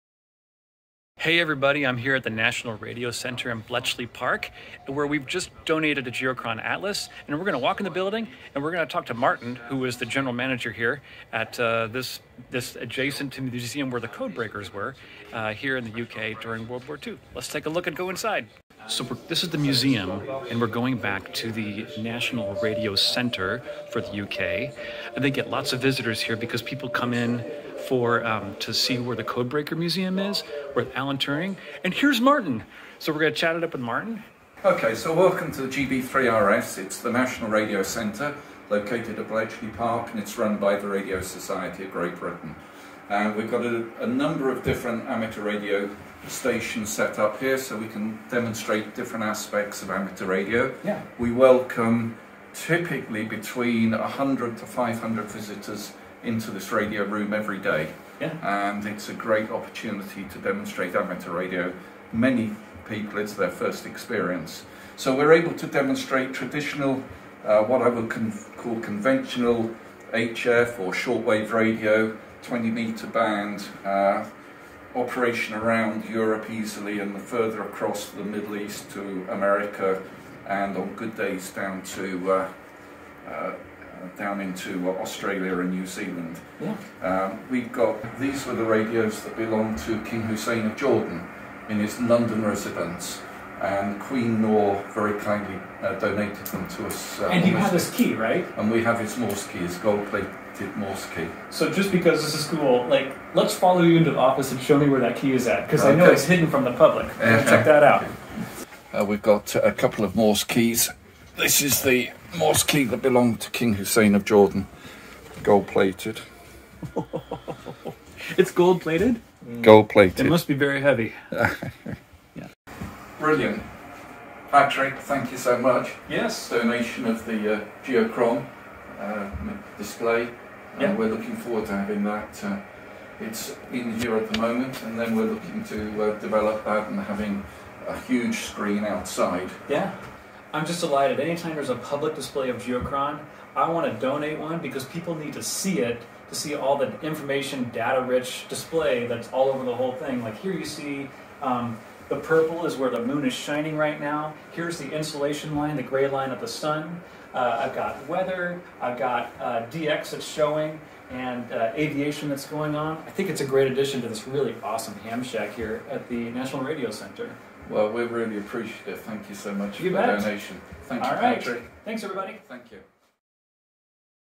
Geochron at Bletchley Park UK with King Hussein's gold plated morse key in the closet!
Patrick at Geochron donates an Atlas to The National Radio Center and finds a gold plated key in the closet.